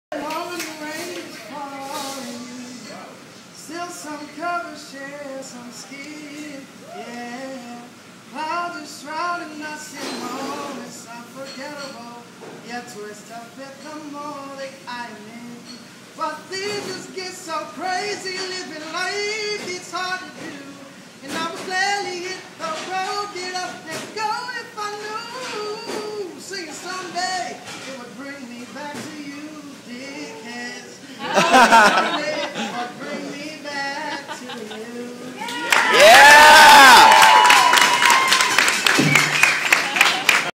Andrew Garcia (American idol) sings at Dicks last resort i
Andrew Garcia from American idol, gets talked into singing for everyone.